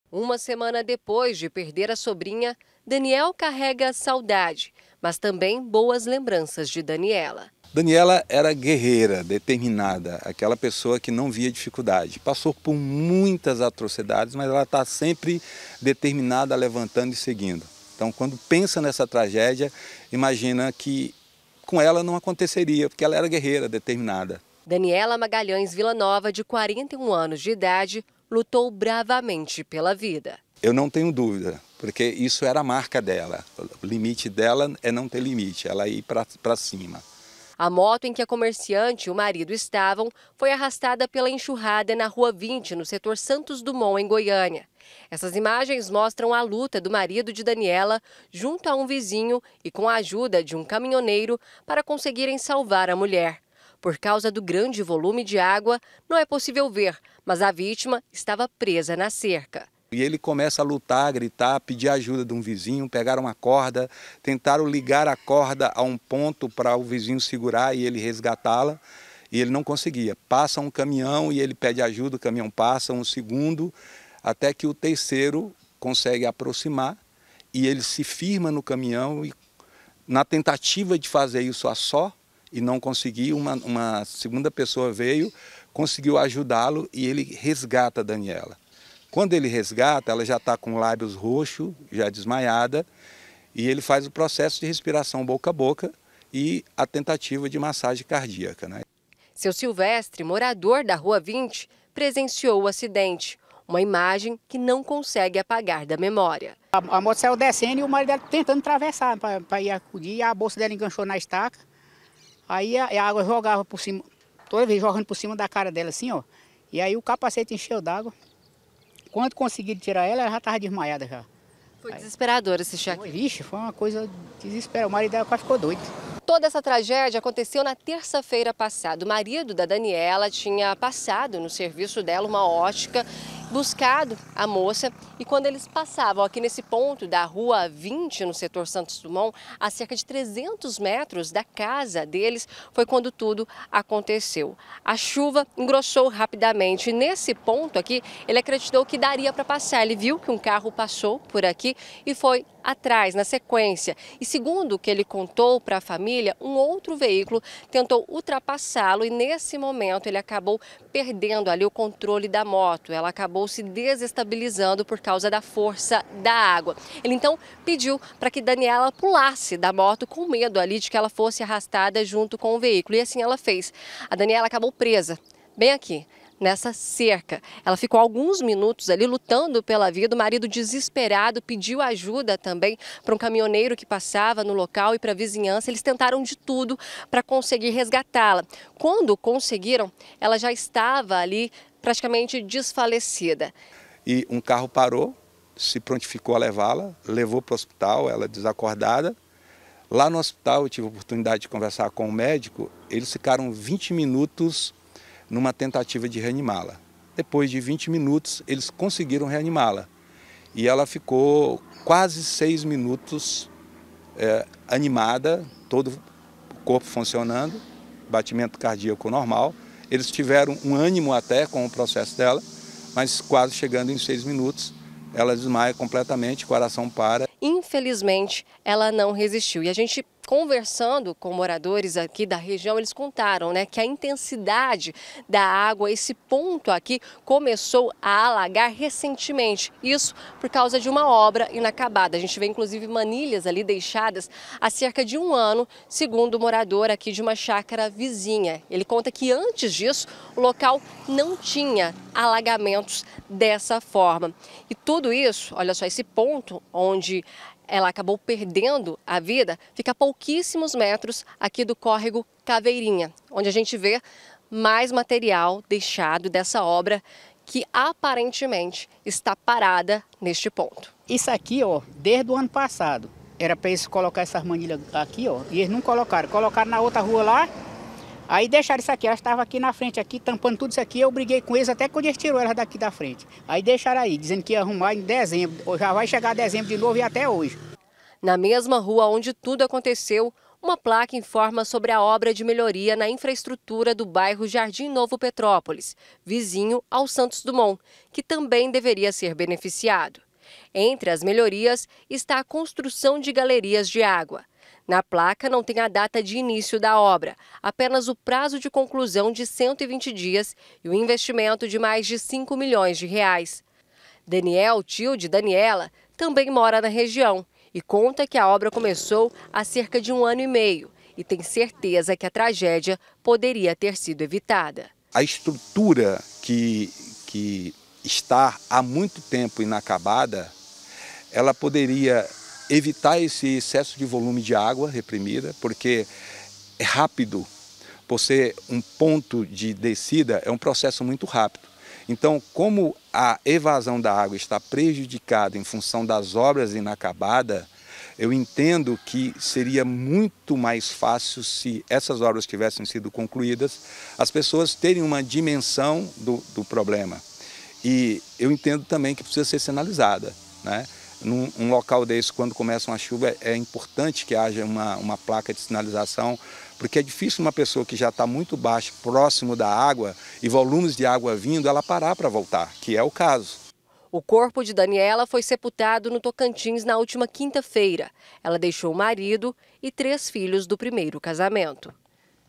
FAMILIARES E VIZINHOS DA VÍTIMA ACREDITAM QUE MORTE PODERIA TER SIDO EVITADA
A FAMÍLIA DA MULHER QUE MORREU DEPOIS DE SER LEVADA PELA ENXURRADA, EM GOIÂNIA, FALOU PELA PELA PRIMEIRA VEZ SOBRE A TRAGÉDIA. DANIELLA MAGALHÃES, DE 41 ANOS, VOLTAVA PRA CASA NA GARUPA DE UMA MOTO, COM O MARIDO, QUANDO FOI ATINGIDA PELO GRANDE VOLUME DE ÁGUA E FICOU PRESA NUMA CERCA. PARA O TIO E VIZINHOS DA VÍTIMA, A MORTE PODERIA TER SIDO EVITADA, COM A CONCLUSÃO DE UMA OBRA QUE ESTÁ PARADA.